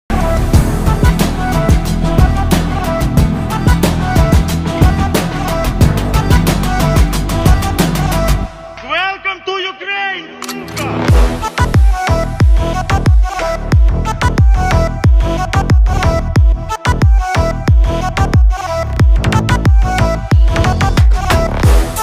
Ukrainian military vehicle
convoy of vehicles of the Ukrainian military forces